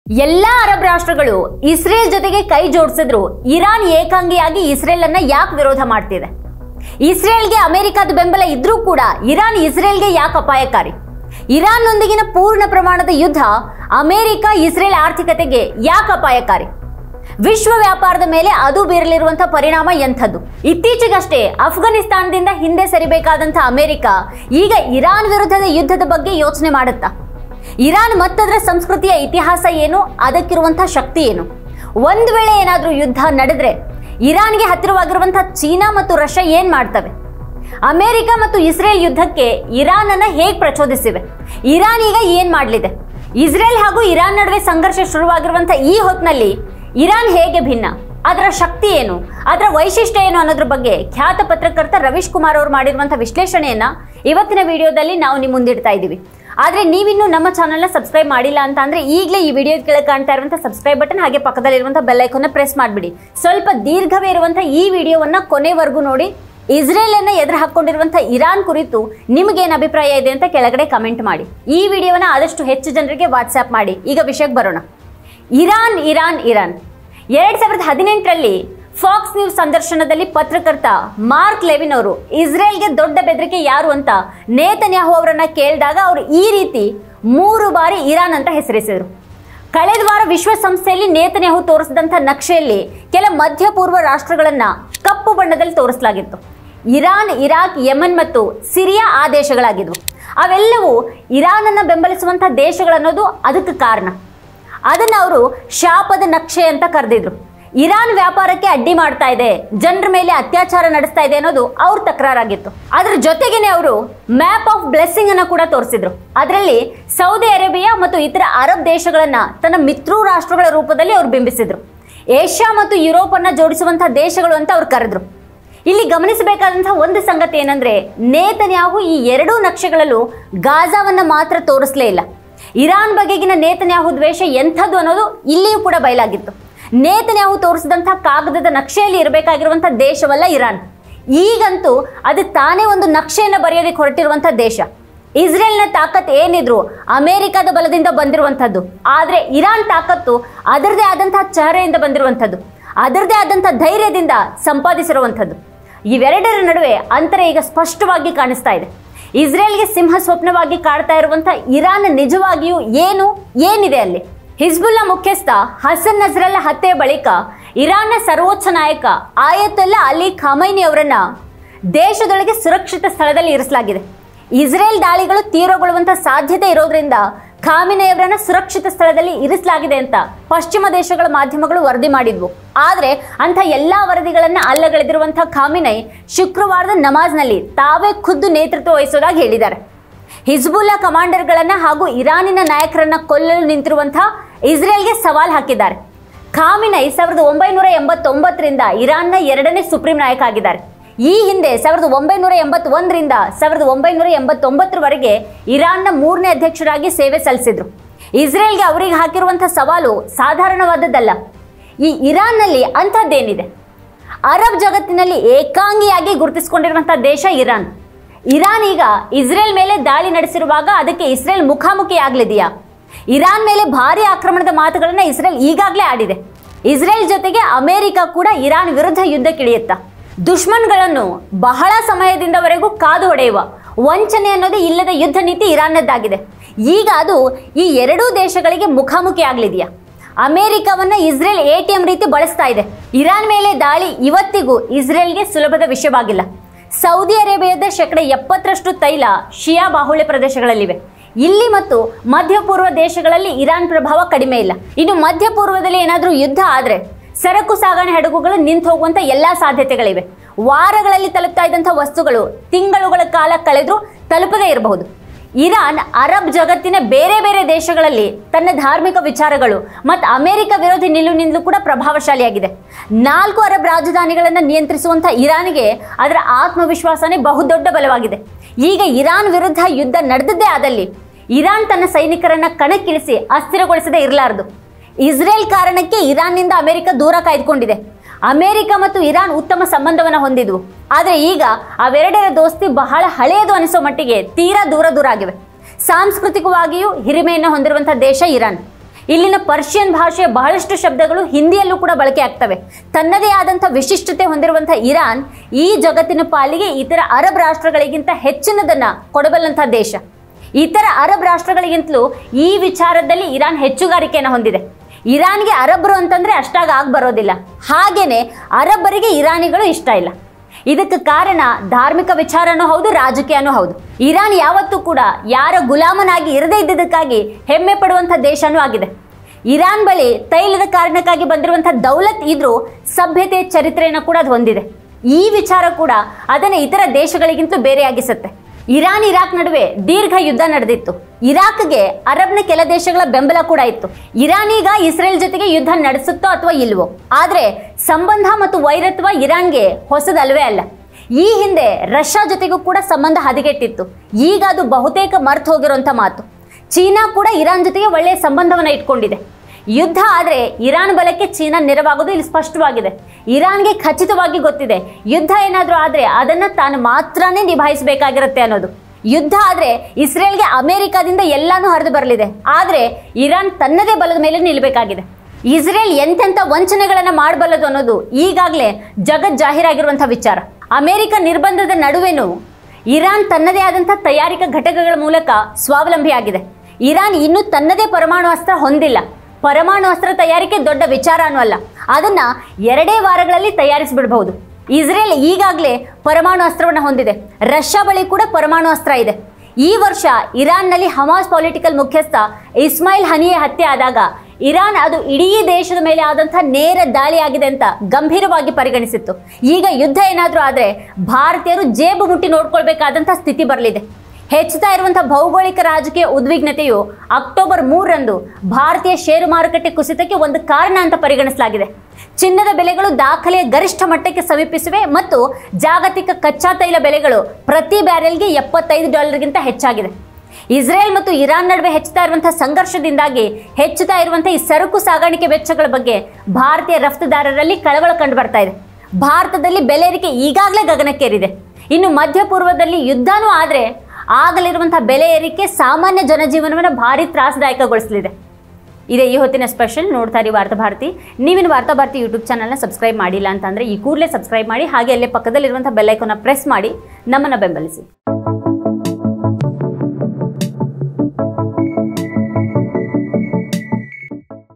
ಇರಾನ್ ಏಕಾಂಗಿಯಾಗಿ ಇಸ್ರೇಲ್ ಅನ್ನು ಏಕೆ ವಿರೋಧಿಸುತ್ತಿದೆ? | Russia | Iran | Israel
ಇರಾನ್‌ ಜೊತೆ ಯುದ್ಧ ಅಮೆರಿಕ-ಇಸ್ರೇಲ್ ಗೆ ಏಕೆ ಅಪಾಯಕಾರಿ?

► ಯುದ್ಧ ನಡೆದರೆ ಚೀನಾ ಮತ್ತು ರಷ್ಯಾ ಏನು ಮಾಡುತ್ತವೆ?